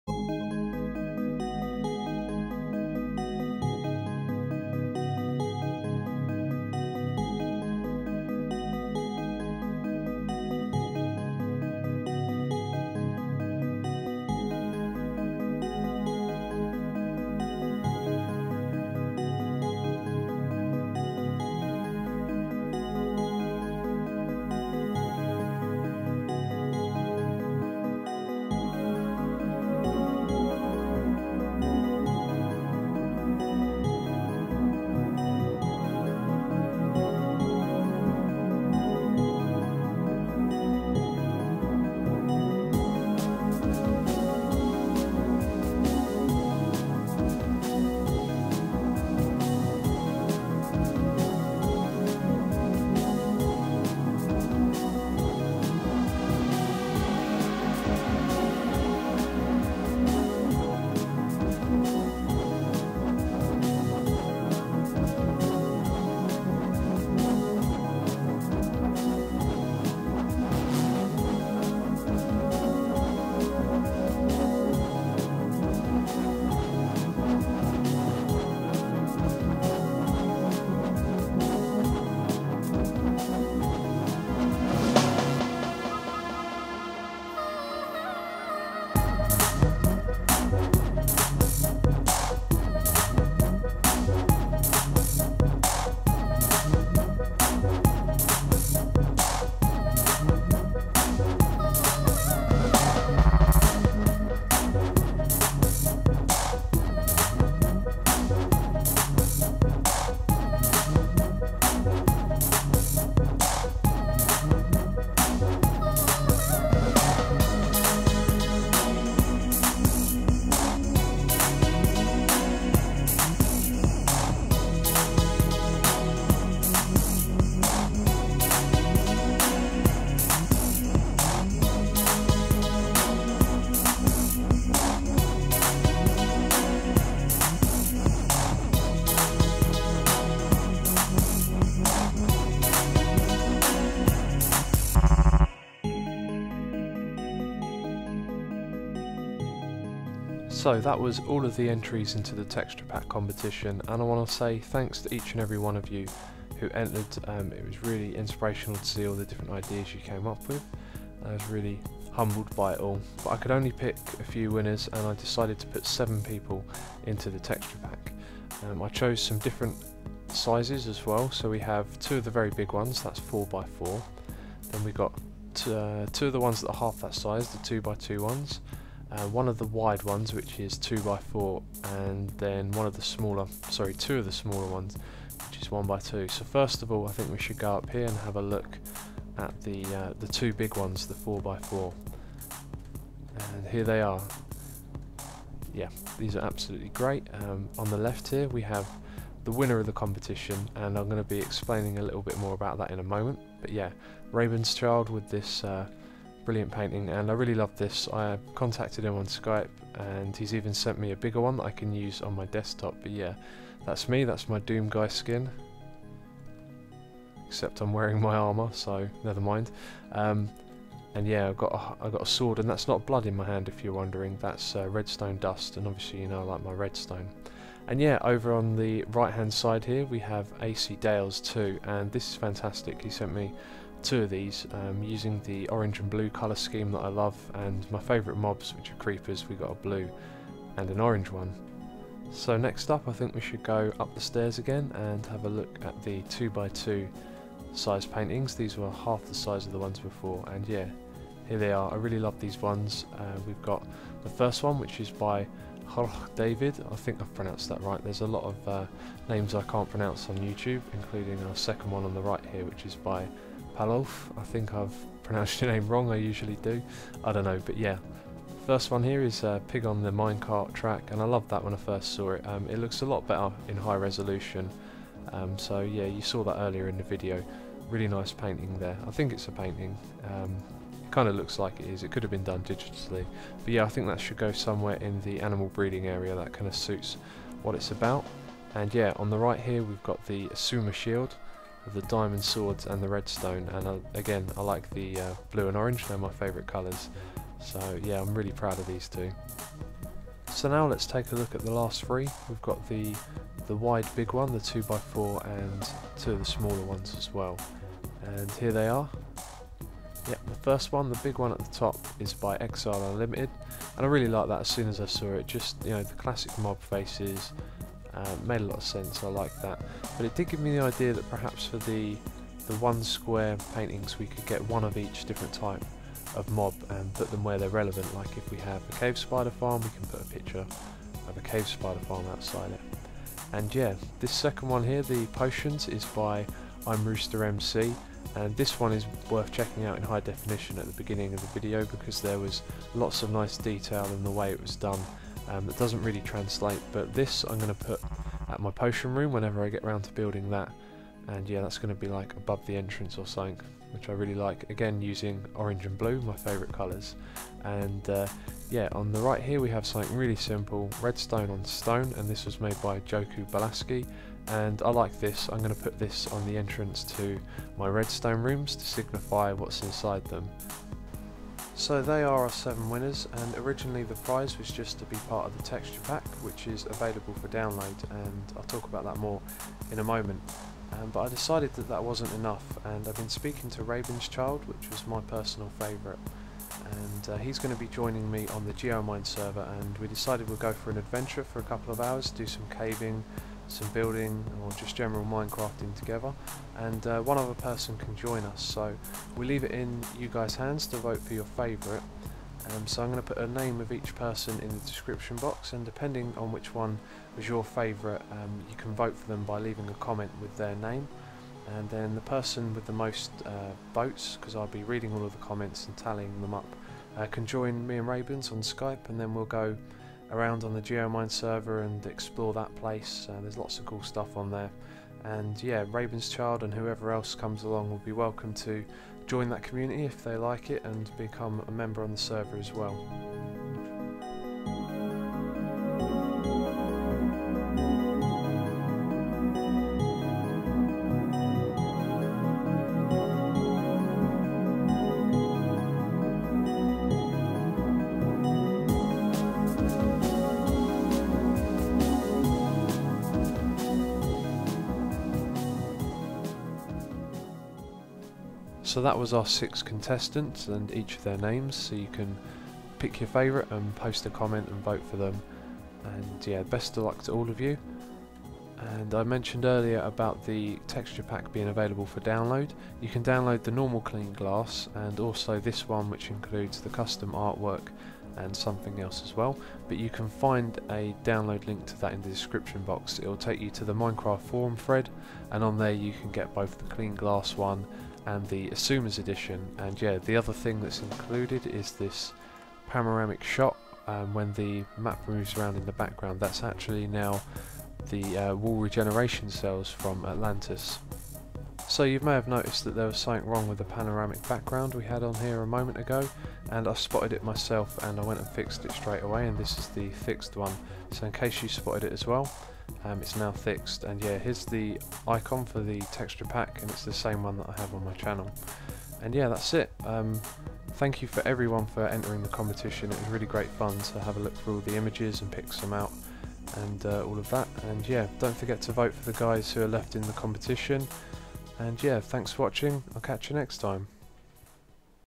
Texture Pack Competition Results
Here are the results for the texturepack competition, dont forget to vote for one of our 6 finalists!

They are:
acdales2
ExileUnlimited
HrochDavid
ImRoosterMC
jokubolakis6
paolovf

I also have a technical / construction LP where i walk you through epic builds and creations